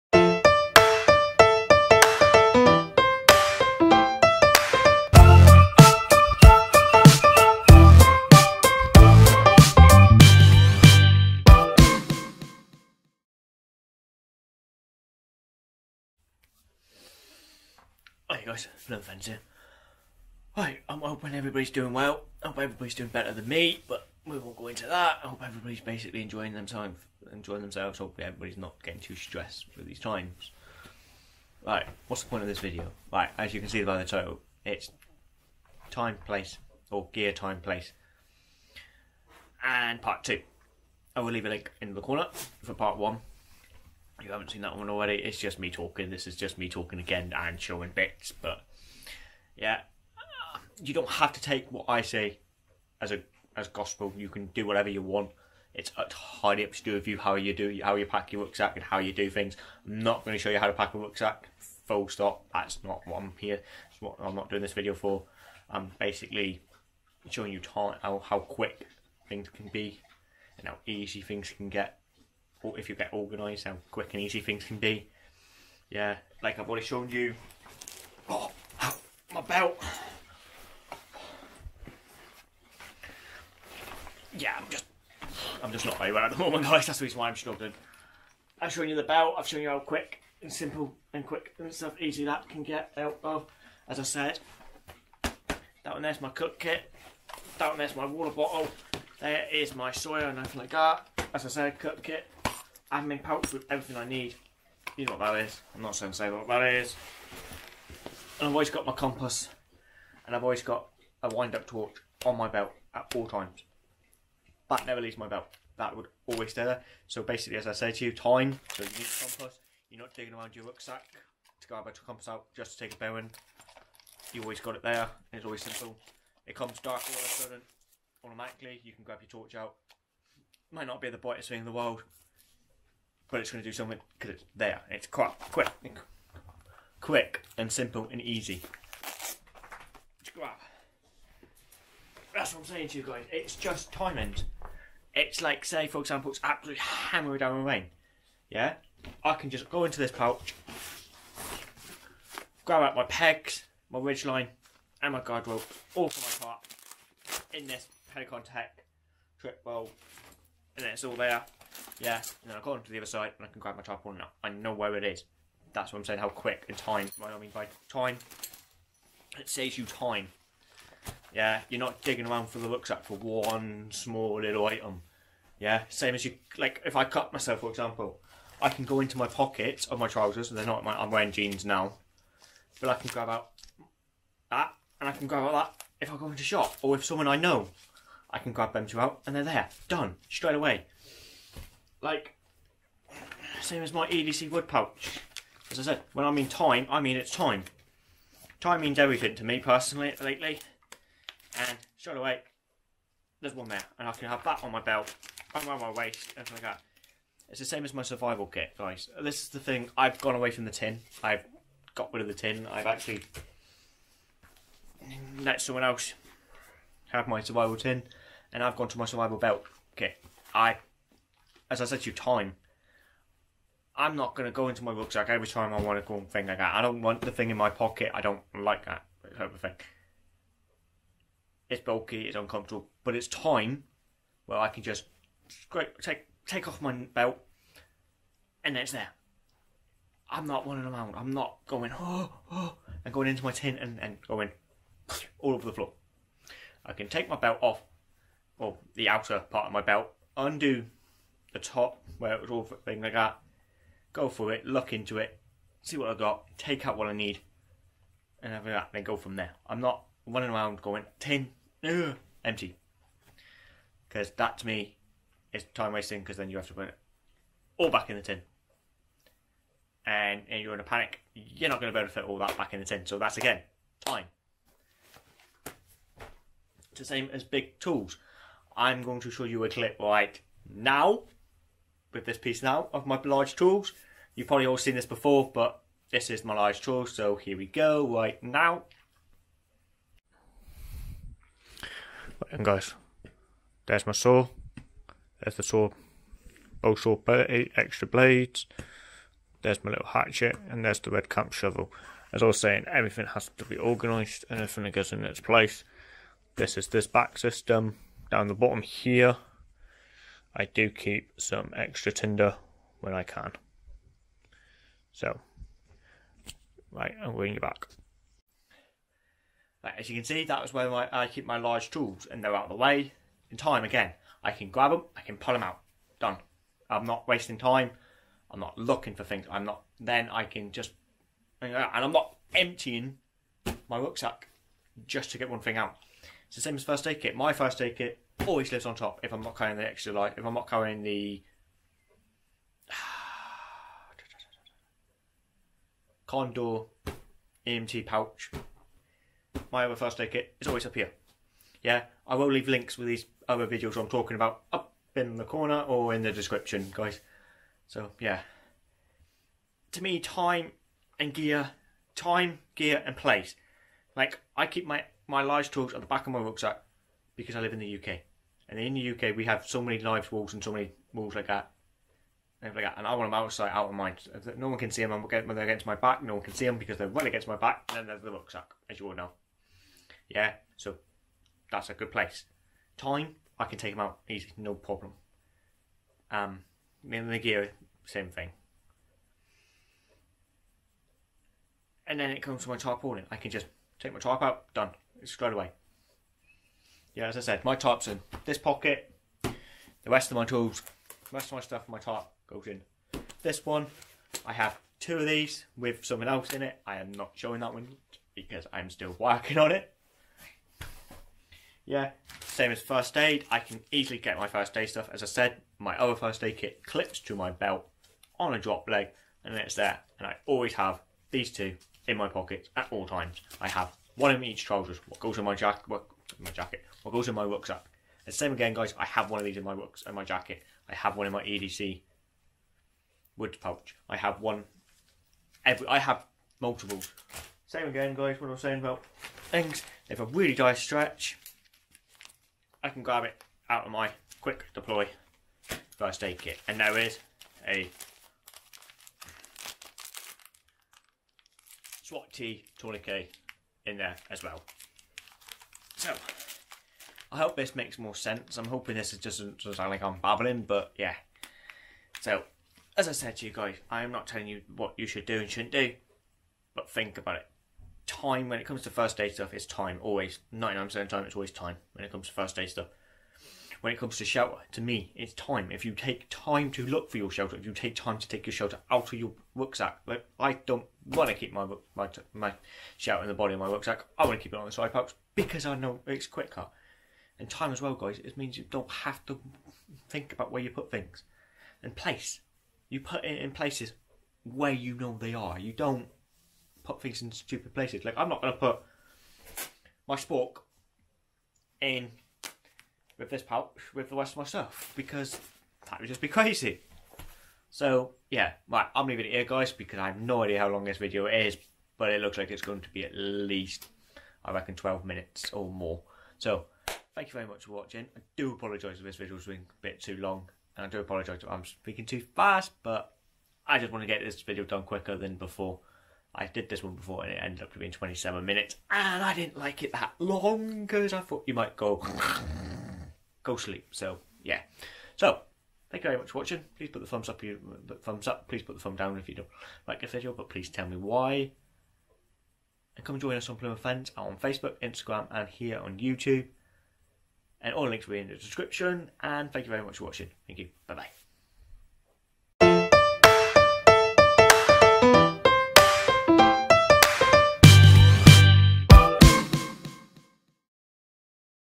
Gear, time and place part two of two
this is part two on Gear and Time and Place and I just try to explain where I like to keep some bit and how fast I can get to them and I think its import and i am not telling you how to you should pack your pack thats up to do with you and thanks for the support and the time you took to watch and comment and please join us here at Plymouth Fenns